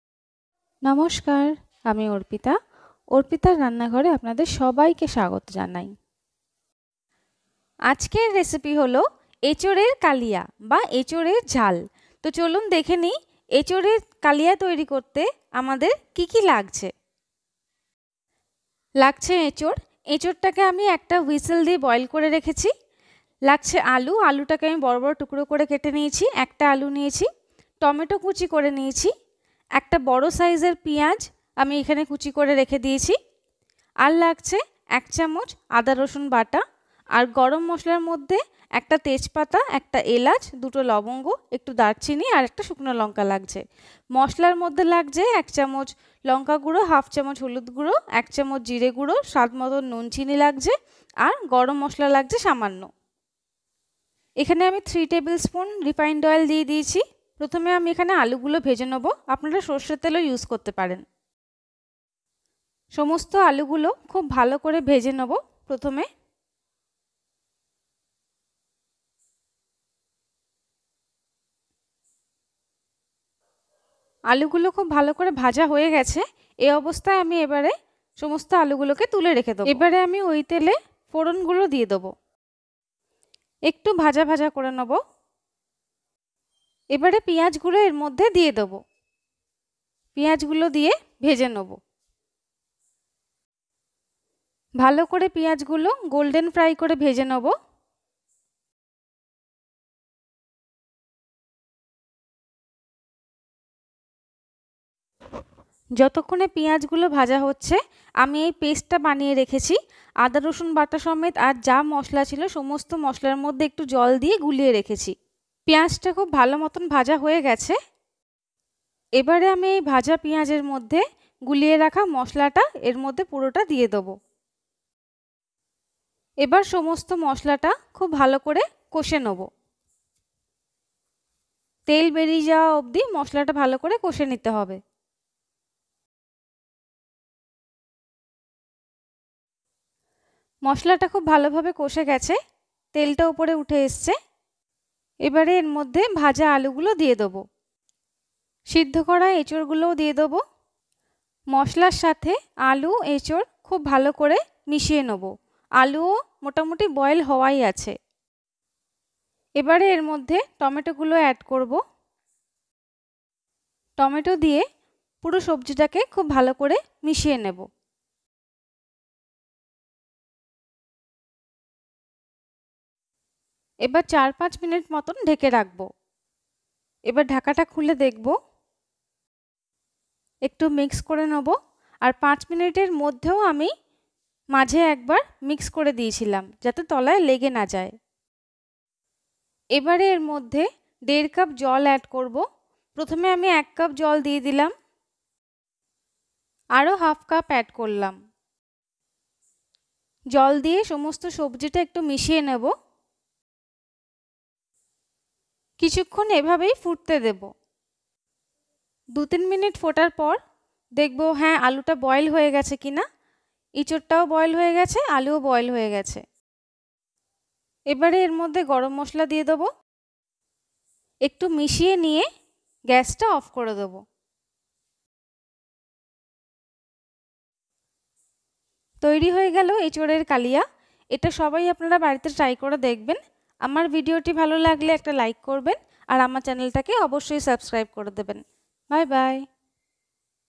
Echor er Kalia | Echor Curry | Arpitar Rannaghar
Ajker ranna Echor er kalia . Khubi sohoje toiri hoe jay. Dekhe nin ami kivabe kori ei echor curry.

Ingredients:
• Echor/Jackfruit
• Potato 1
• Onion 1
• Tomato 1
• Bay leaf 1
• Cardamom 1
• Cloves 2
• Turmeric powder half tablespoon
• Cumin powder 1 tablespoon